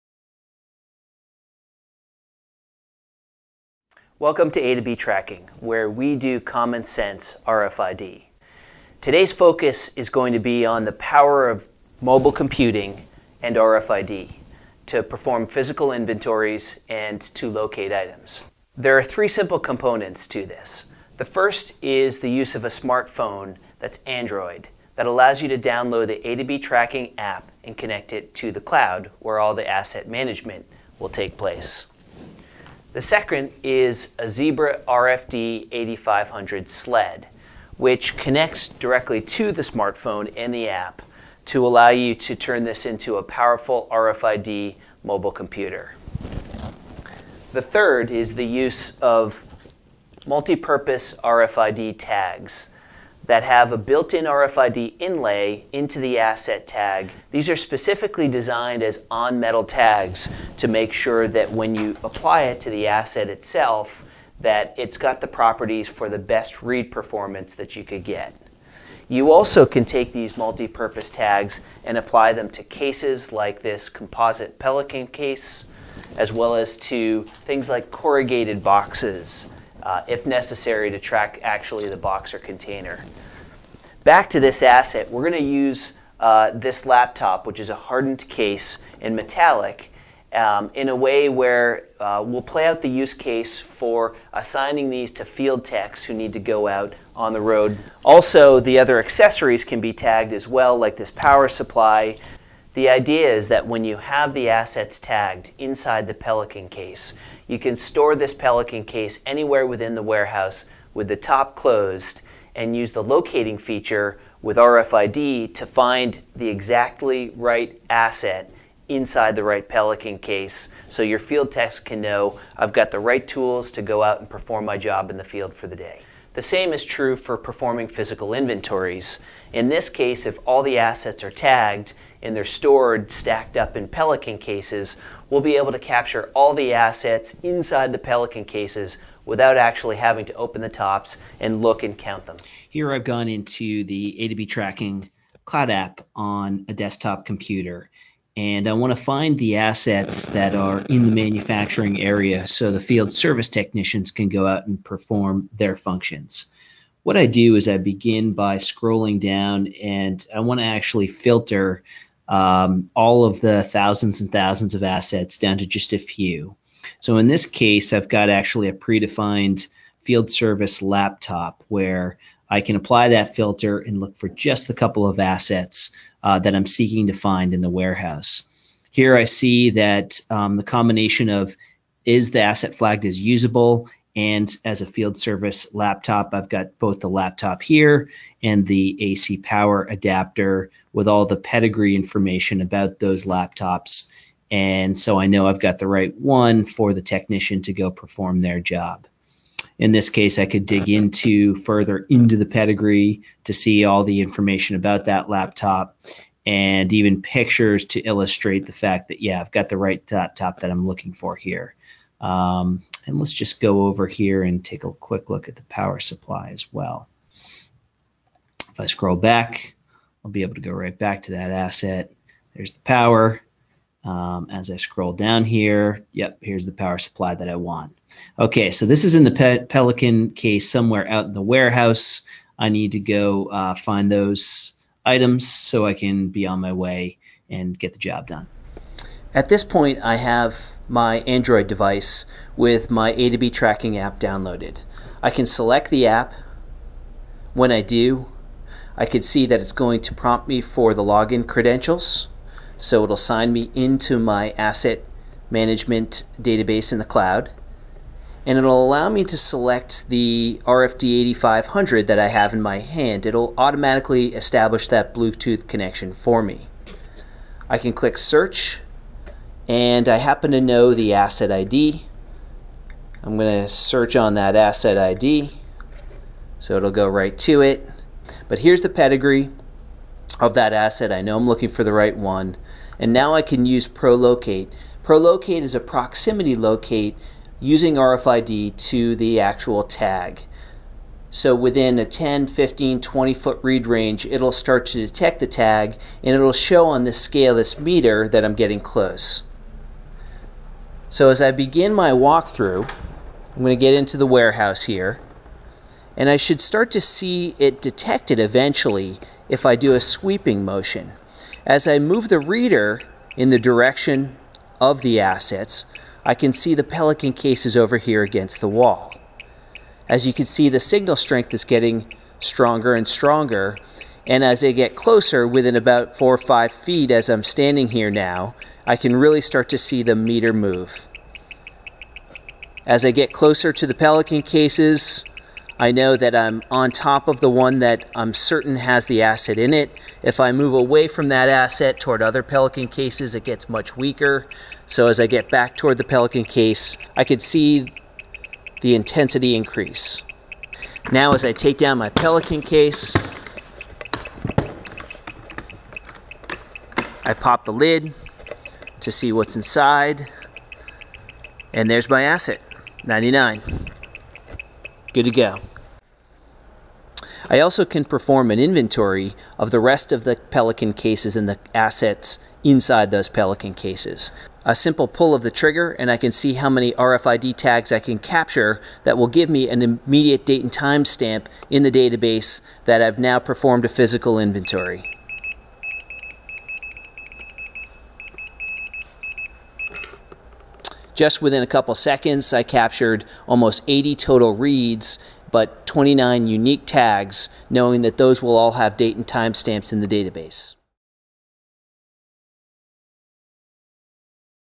RFID Mobile Asset Tracking Demo
Watch this quick overview demonstration of A2B Tracking's powerful Mobile RFID capabilities. CEO and President, Peter Collins walks you through a use case scenario where he performs a physical inventory and locates specific assets using mobile RFID technology.